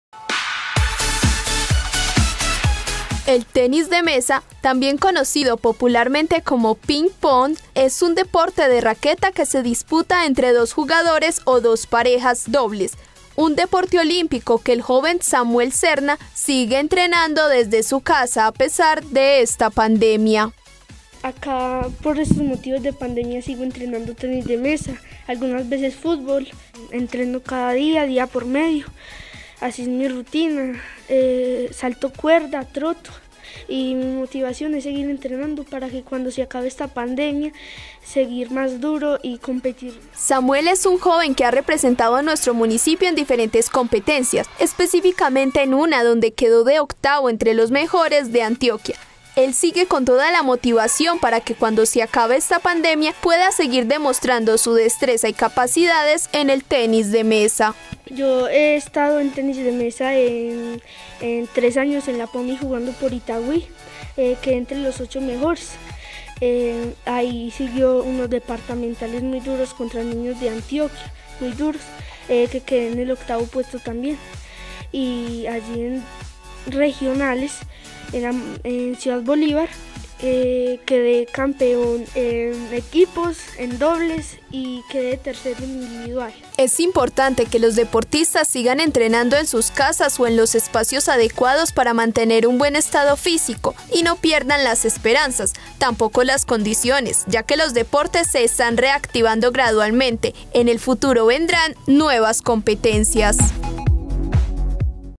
Entrenamiento y disciplina desde casa
Samuel Serna aprovecha el receso educativo y la cuarentena para seguir entrenando en casa, la disciplina de tenis de mesa su principal pasión.